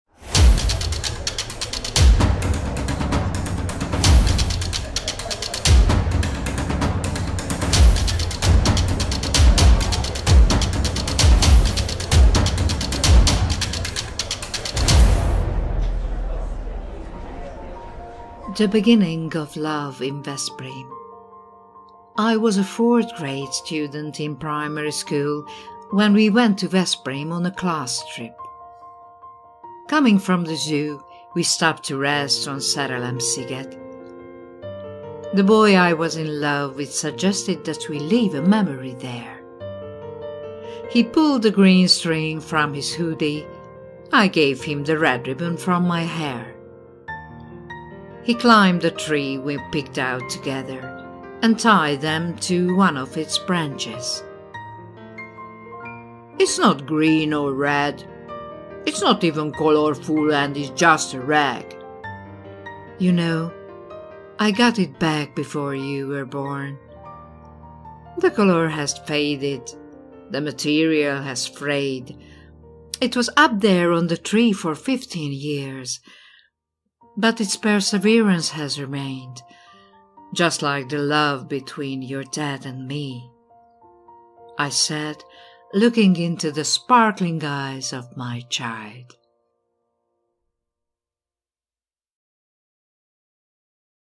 PART 12. VEB2023 - WELCOME WORLD! VESZPRÉM IN 100 WORDS Come and meet us in 100 words!
VEB2023 - WELCOME WORLD! VESZPRÉM IN 100 WORDS, THE TRUE STORIES
Come and meet us in 100 words!
Part 12.

Nr. 109 novel: The beginning of love in Veszprém (Author: Nency Walker)

I was a 4th grade student in primary school when we went to Veszprém on a class trip. Coming from the zoo, we stopped to rest on Szerelem-Sziget. The boy I was in love with suggested that we leave a memory there. He pulled the green string from his hoodie, I gave him the red ribbon from my hair. He climbed a tree we picked out together and tied them to one of its branches. "It's not green or red, it's not even colourful and it's just a rag." "You know, I got it back before you were born. The colour has faded, the material has frayed, it was up there on the tree for 15 years, but it's perseverance has remained, just like the love between your dad and me," I said, looking into the sparkling eyes of my child.

100 szóban Veszprém történetíró pályázat /2022

109. történet: Szerelem kezdete Veszprémben (Szerző: Nency Walker)
Általános iskolai 4. osztályos tanuló voltam, amikor Veszprémben jártunk osztálykiránduláson. Az állatkertből jöttünk, megálltunk a Szerelem-szigeten pihenni. A fiú, akibe szerelmes voltam, mondta, hogy hagyjunk ott egy emléket. Kihúzta a kapucnija zöld madzagját, én odaadtam a hajamból a piros szalagot. Felmászott egy közösen kiszemelt fára, és az egyik ágára felkötötte őket. - Ez nem zöld vagy piros, nem is színes, és tiszta rongyos. Tudod, születésed előtt kaptam vissza. A színe megkopott, az anyag foszlott, 15 évig volt ott fent a fán, de a kitartása megmaradt, akárcsak apukáddal a szerelmünk. - mondtam a csillogó tekintetű gyermekem szemébe nézve.